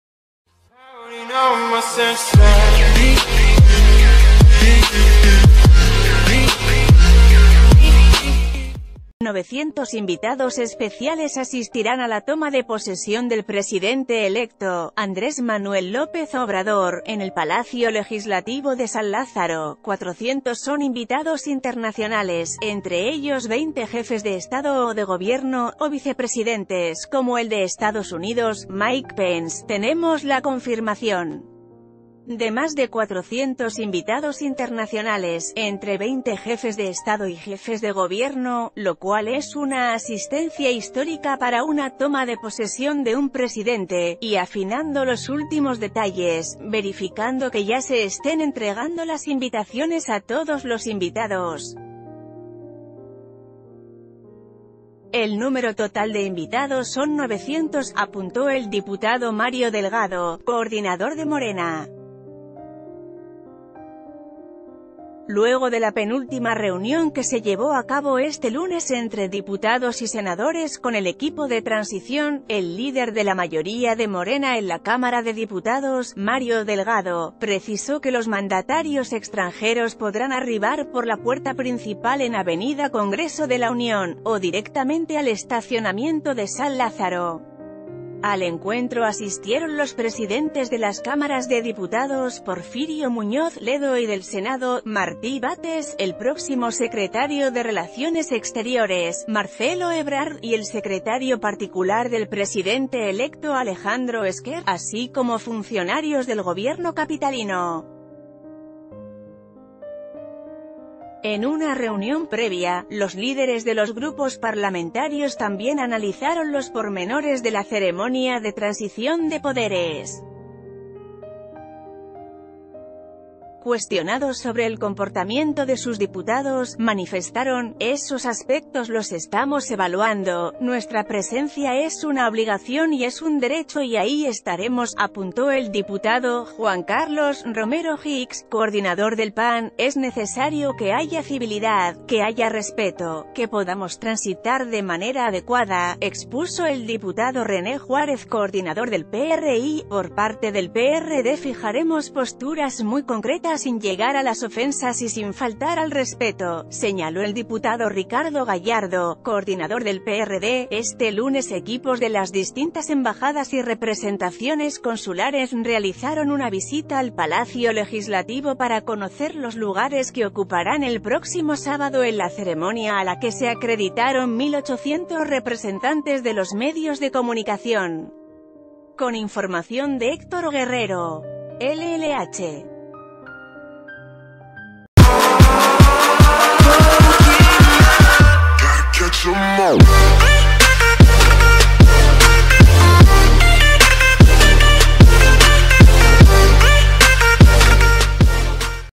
Confirmados, 900 invitados a la toma de posesión de AMLO
Novecientos invitados especiales asistirán a la toma de posesión del presidente electo,  Andrés Manuel López Obrador, en el Palacio Legislativo de San Lázaro; 400 son invitados internacionales, entre ellos 20 jefes de Estado o de Gobierno, o vicepresidentes, como el de Estados Unidos, Mike Pence.“Te...